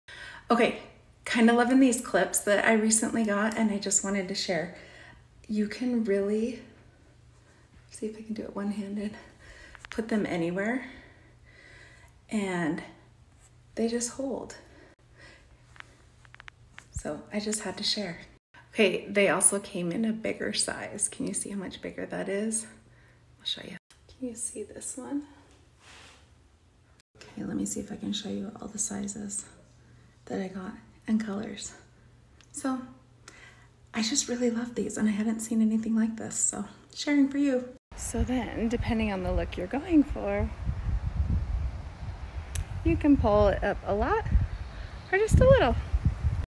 Cute duckbill hair clips
Black and brown clips in 2 sizes.

So great for curly hair or straight hair!!